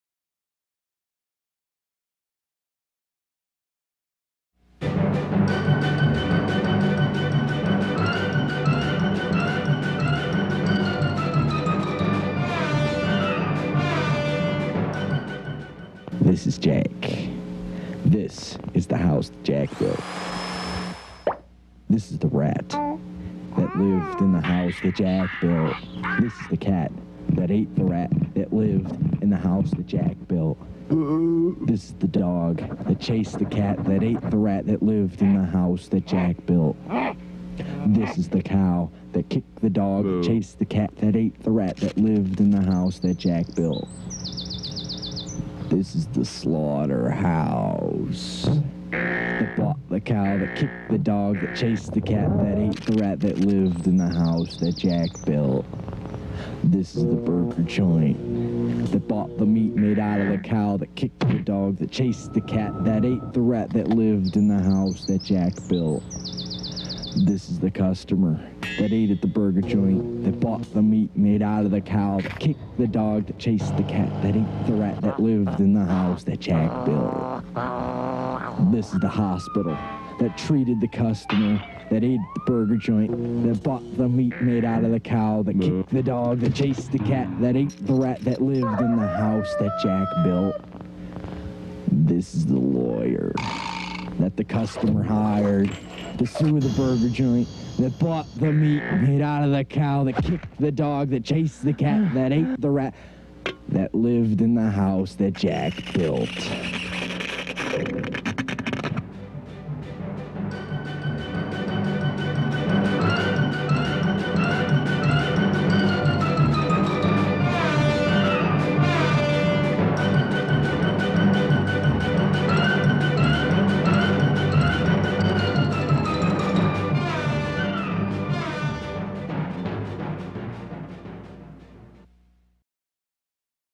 The House That Jack Built
This was made way back in 1992 on an Amiga 3000. Yes, you read that right. On an Amiga 3000. With Deluxe Paint, even. I ran across an old project tape and found this on there and figured I post it for all the world to see. Enjoy!